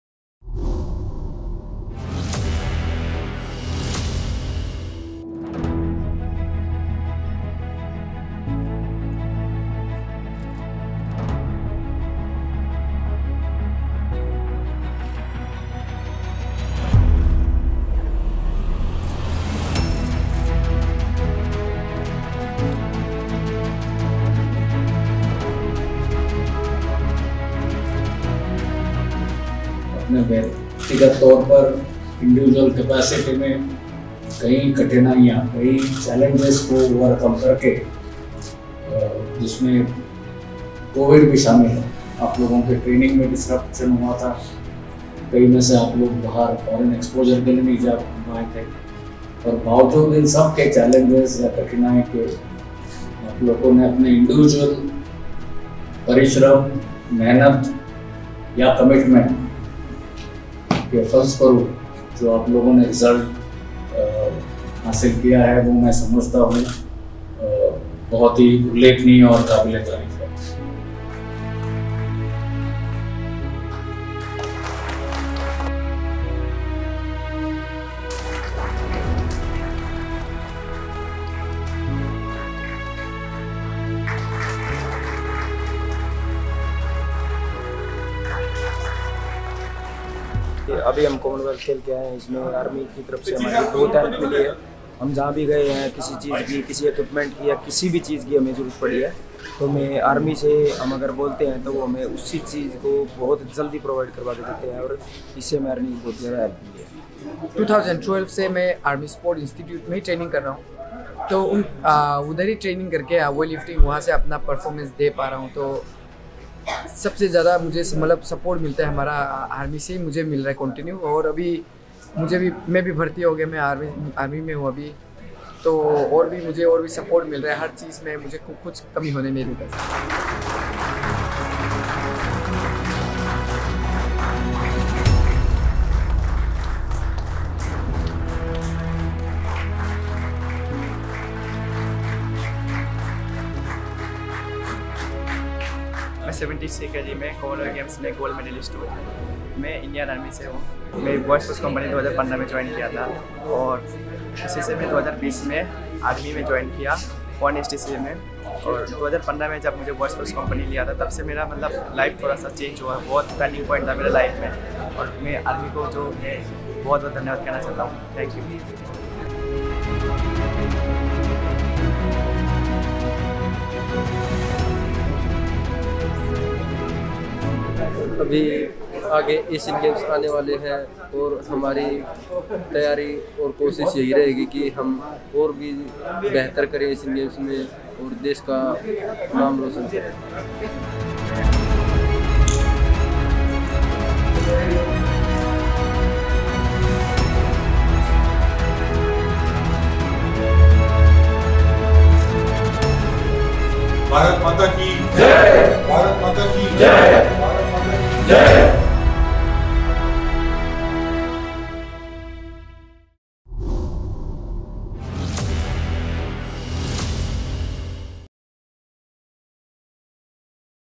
Indian Army Celebrates Commonwealth Games Medal Winners
Army Chief, General Manoj Pande, felicitated the Indian Army’s sportspersons who participated in the Commonwealth Games in the UK from July 28 to August 8.  On the return of the team to India, Gen Manoj Pande and senior officers of the Army HQ congratulated and interacted with the sportspersons at an exclusive function organised at Delhi Cantt.

Out of 18 participants from the Indian Army in the Commonwealth Games squad, eight sportspersons earned medals clinching four Gold, one Silver, and three Bronze Medals. These medals are a result of a carefully planned and sustained “Mission Olympic Programme” by the Indian Army, which was conceived and implemented in 2001.

The Army Chief felicitated the participants by awarding Commendation Cards and cash incentives.
Addressing the sportspersons, Gen. Pande acknowledged their superlative performance. The Indian Army participants are the real role models for the nation, he added. He said that their individual performances have made the Indian Army and the entire Nation proud, and wished them the very best for all the forthcoming events.